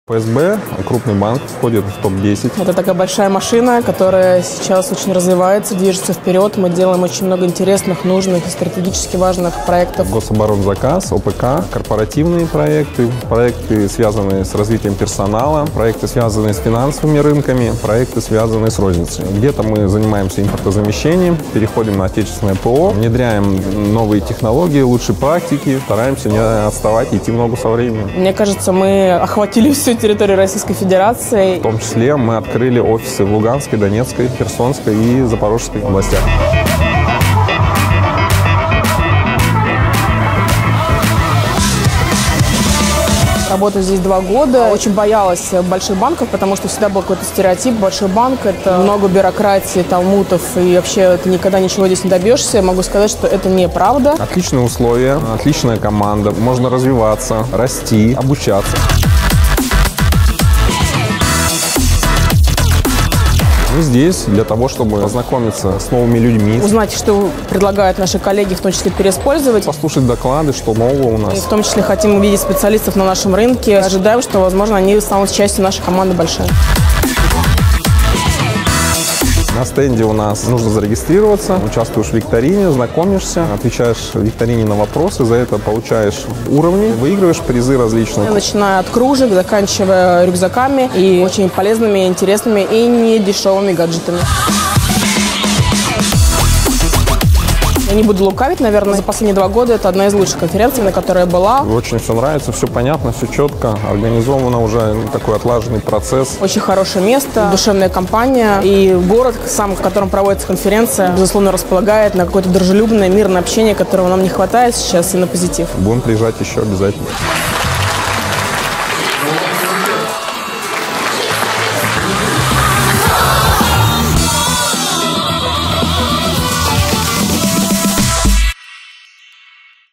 Промсвязьбанк. Партнёр конференции Analyst Days #15.
Промсвязьбанк. Серебряный партнёр конференции Analyst Days #15.

Analyst Days является конференцией, на которой обсуждается ряд профессиональных вопросов, среди которых: последние инновации, тенденции, опыт, результаты и проблемы в области проектирования требований, практика управления требованиями, необходимые навыки, техники, технологии, инструментарий и т.д. Конференция будет интересна всем без исключения специалистам, вовлеченным в процессы работы с требованиями.